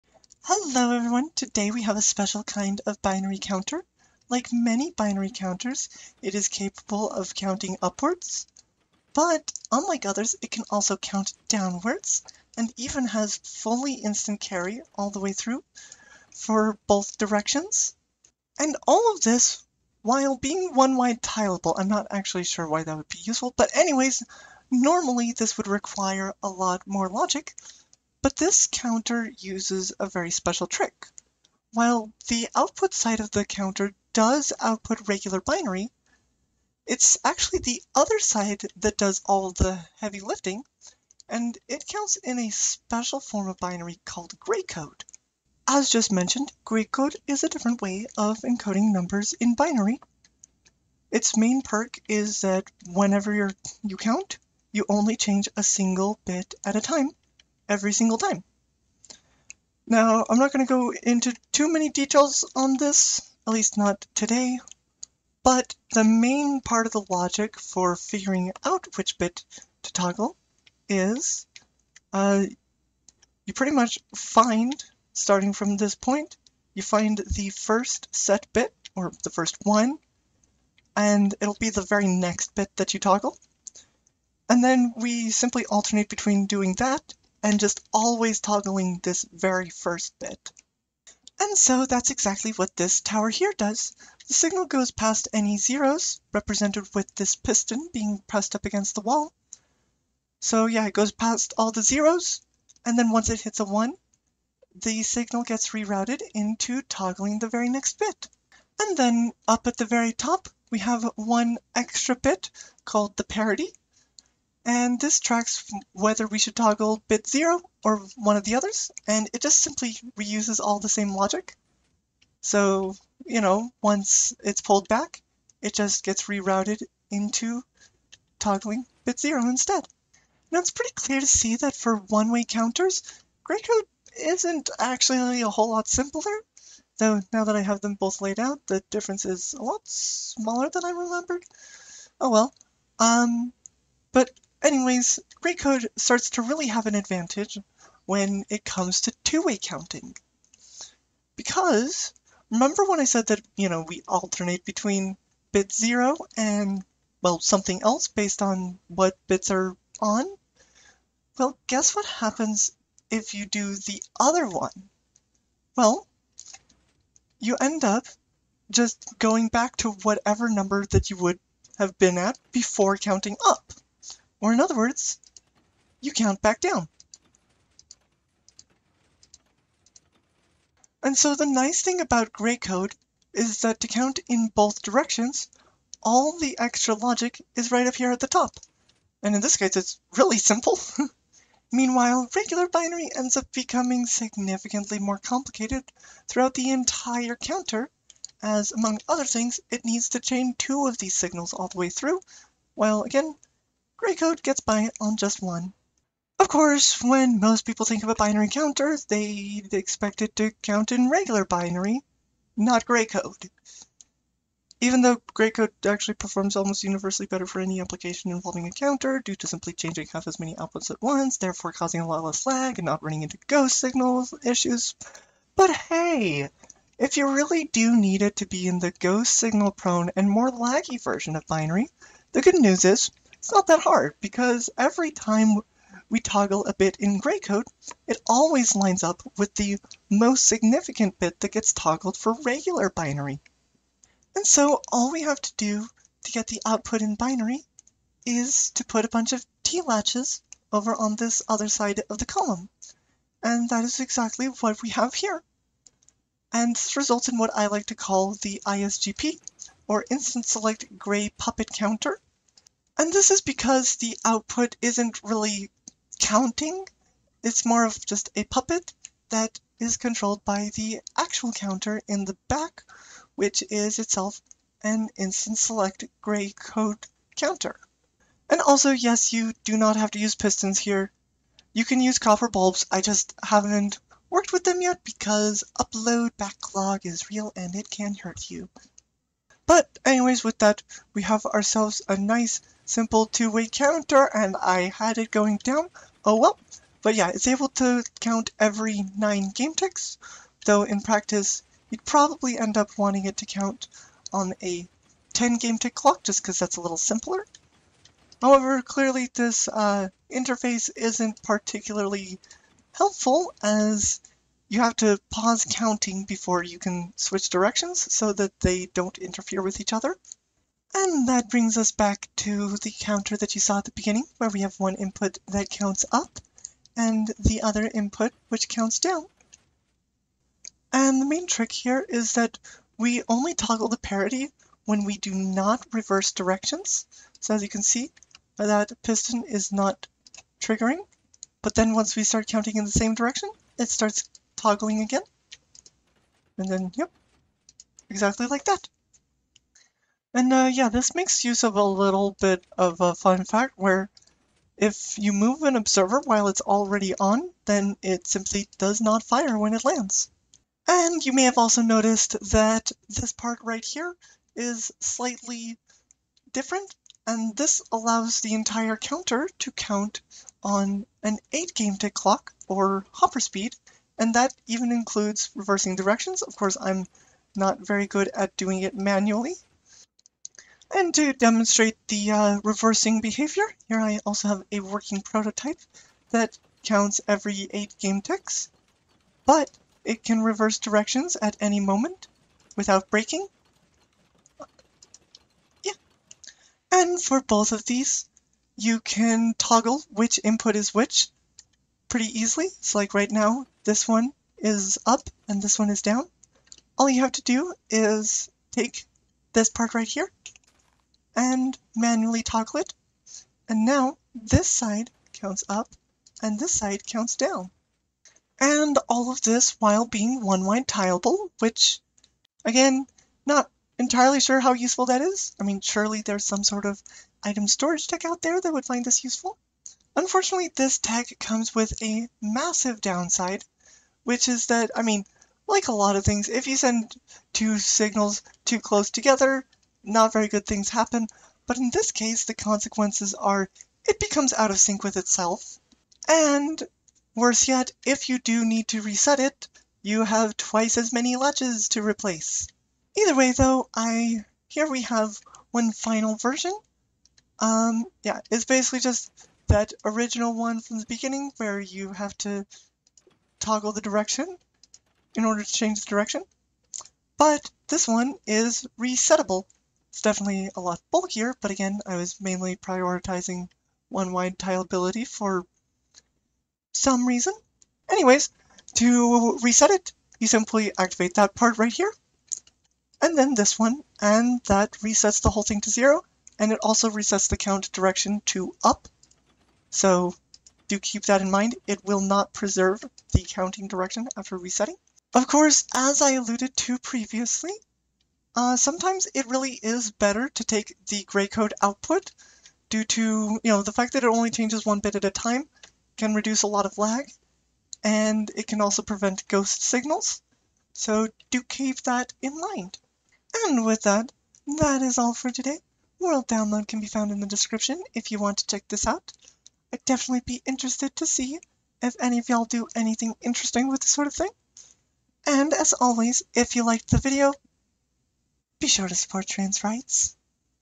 [Java] 1-wide 2-way Instant Carry Binary Counters (and Gray Code too!)
2 way counters that use far less logic than traditional means, more or less. this is achieved by first counting in Gray code, then using the Gray code counter to count in weighted binary (the usual form of binary). Gray code is so much simpler for 2 way counting that this ends up being more compact.

previous (much more drawn out) video on the same subject, but on bedrock, and the goal was speed instead of compactness

chapters:
0:00 intro
0:45 Gray code
3:26 ISGP
5:20 improved interface